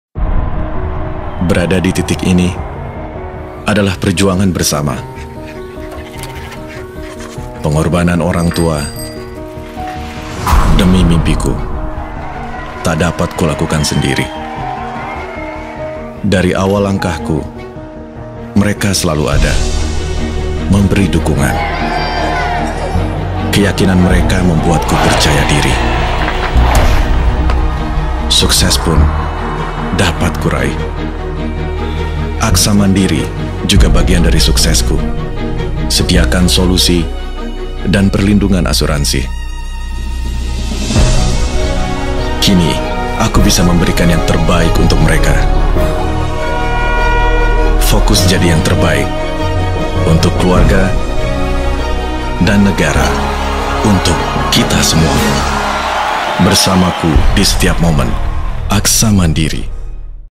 AXA Mandiri, Bersamaku di Setiap Momen
Dari awal yang sederhana hingga menjadi juara dunia, cerita Eko berkisah tentang keyakinannya pada diri sendiri. Eko didukung oleh keluarga, teman, pelatihnya dan AXA Mandiri. Di setiap langkahnya, kita dapat menyaksikan sebuah cerita suskses yang luar biasa.